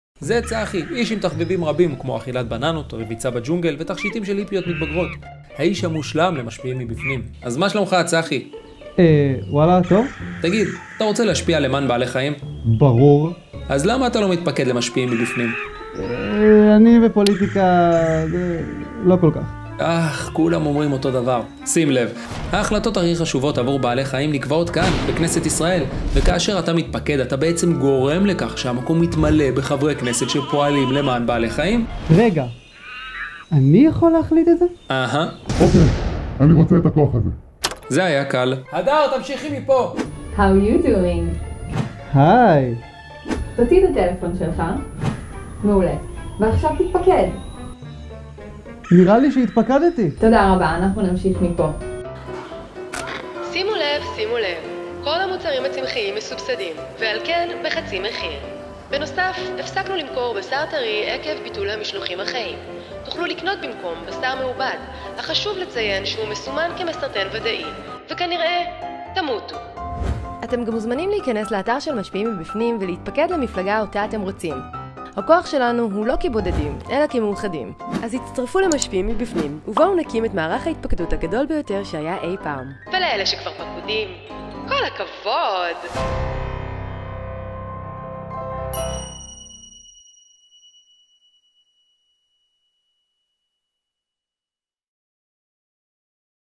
רוצים להשפיע למען זכויות בעלי חיים? פשוט הצטרפו למשפיעים מבפנים
אתם רוצים להשפיע למען זכויות בעלי חיים? פשוט הצטרפו למשפיעים מבפנים
רוצים לשמוע עוד? לחצו כאן

שלוש שניות שלכם שמשפיעות על חיים שלמים. לקול שלכם יש כוח, ורק איתכם נוכל להעביר חקיקות שישנו את מצבם של בעלי החיים בישראל.

פעולה קטנה שלכם, ובום, אתם חלק מהמהפכה.

משפיעים מבפנים, למען בעלי החיים